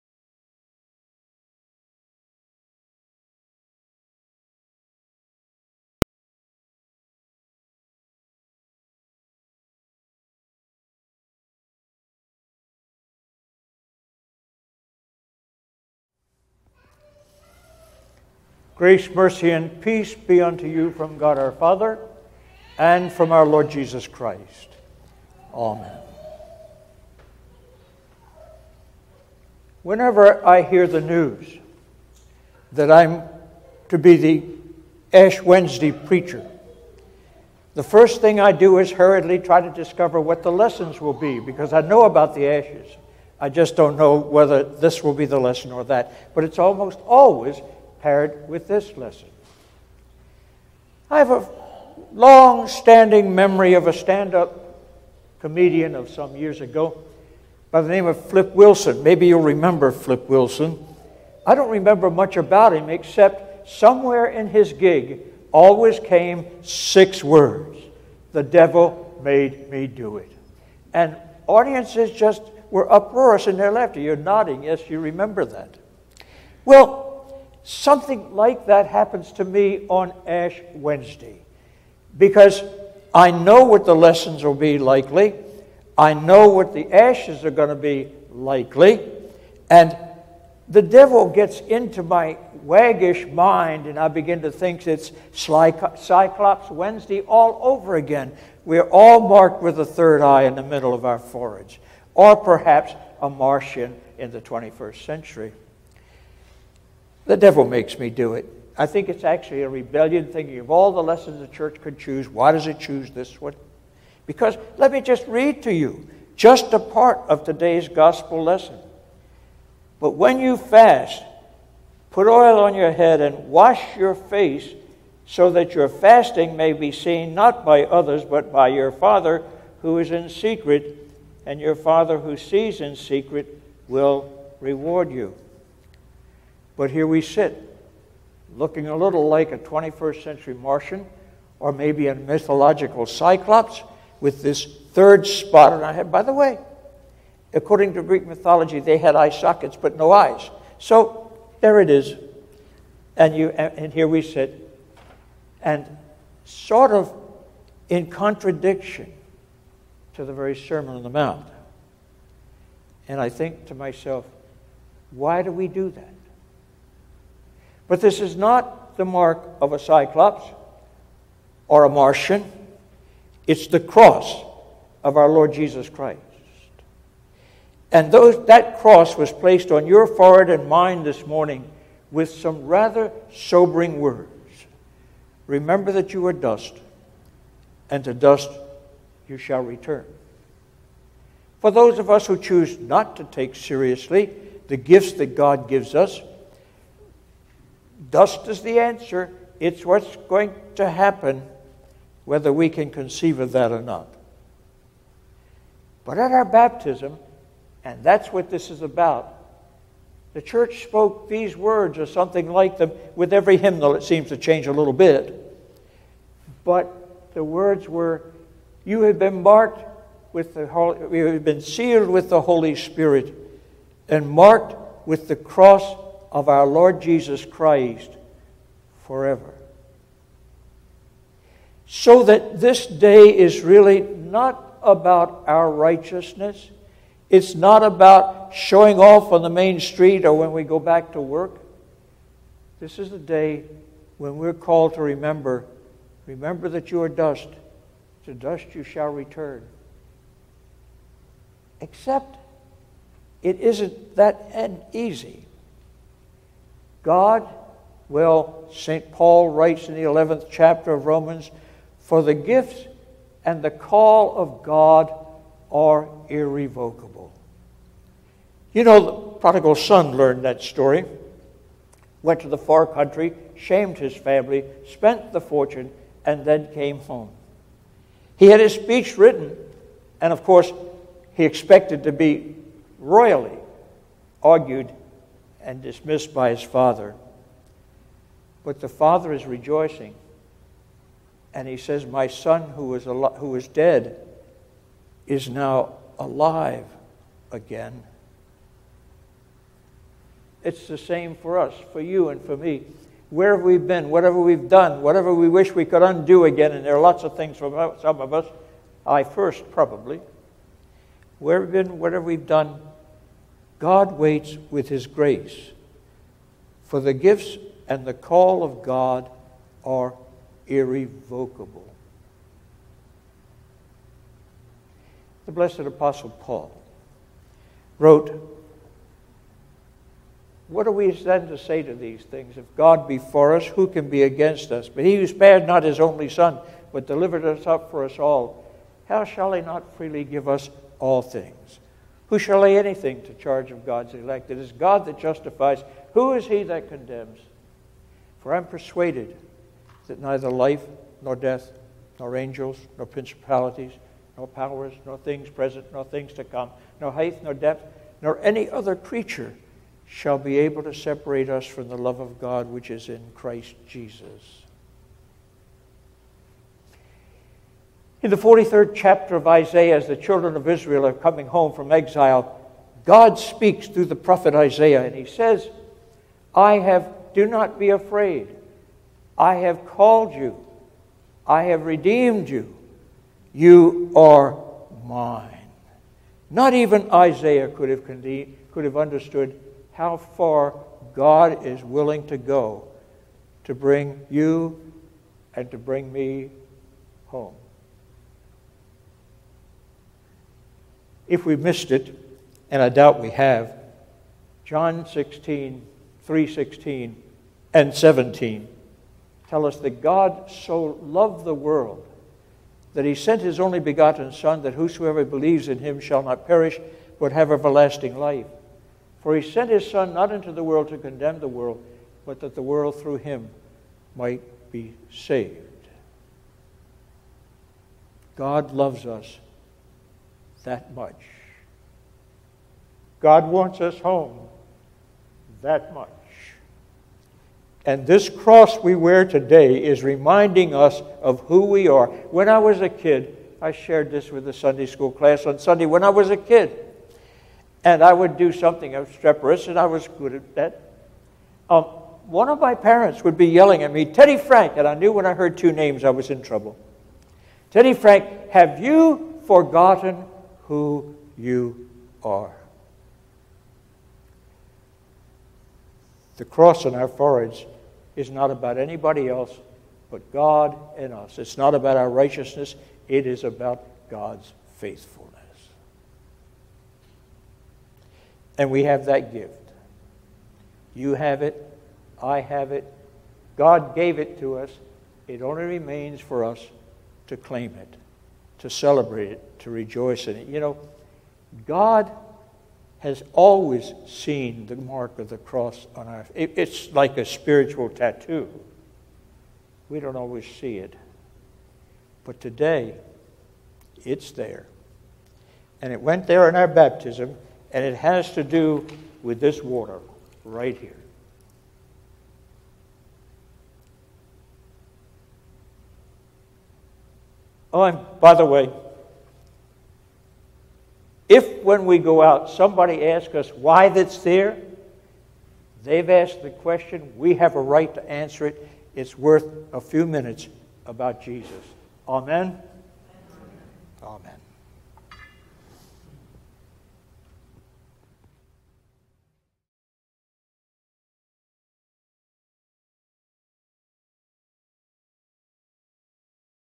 GSLC SERMON ASH WEDNESDAY 02262020
Pastor Ted Schneider reflects on the reasons for getting an ashen cross on our foreheads on Ash Wednesday.

The message for this Ash Wednesday is based on the reading from the Gospel of Matthew 6:1-6,16-21.

Recorded Wednesday, February 26, 2020 at 12:00pm Ash Wednesday Worship Service at Good Shepherd Lutheran Church, Gaithersburg, MD.